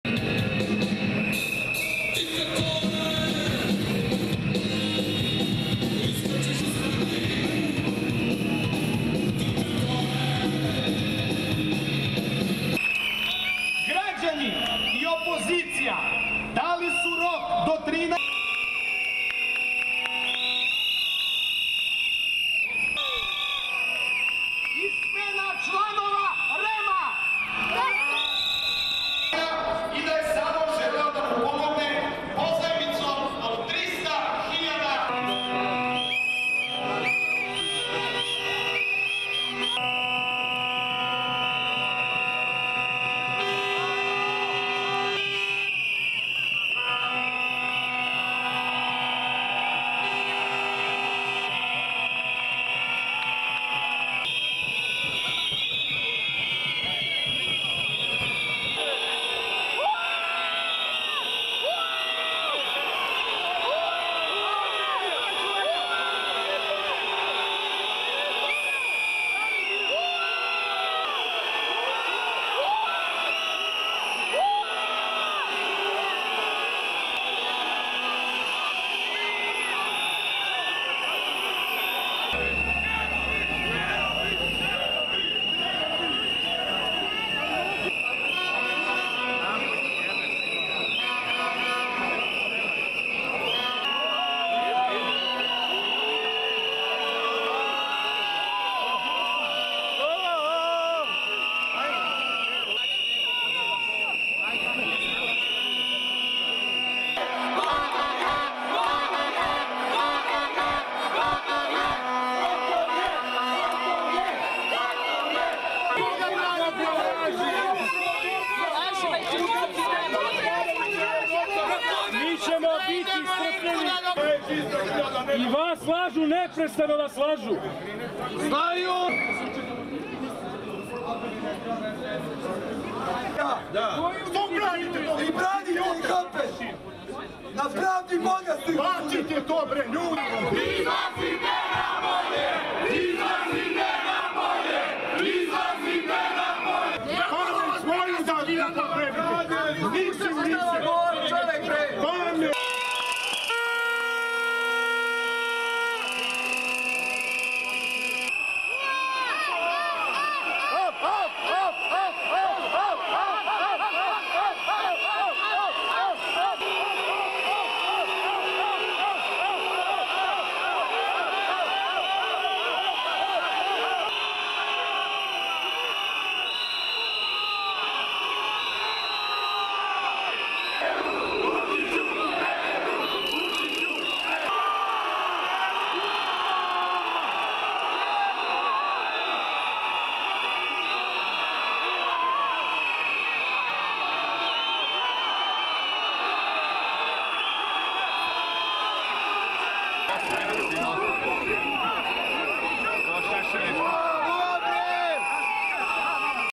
Protesters break into TV Station
(16 Mar 2019) Demonstrators protesting the autocratic rule of Serbian President Aleksandar Vucic burst into state-run TV headquarters in Belgrade on Saturday to denounce a broadcaster whose reporting they consider highly biased.
Helmeted and baton-wielding riot police arrived at the scene in the Serbian capital and tried to evict hundreds of people, including some opposition leaders, who had entered the downtown building.
Some protest leaders said they won't leave until they are given time on the main evening news.
There were reports of injuries during the scuffles.
Thousands of opposition supporters who gathered in front of the building chanted to the police officers: "Shame on you, arrest Vucic!"
Serbian Interior Minister Nebojsa Stefanovic warned the protesters to leave the TV building or police would move in to "protect all Serbian TV journalists."
This was the first major incident after months of peaceful protests against populist leader Vucic.
The demonstrators are demanding his resignation, free elections and media, and more democracy.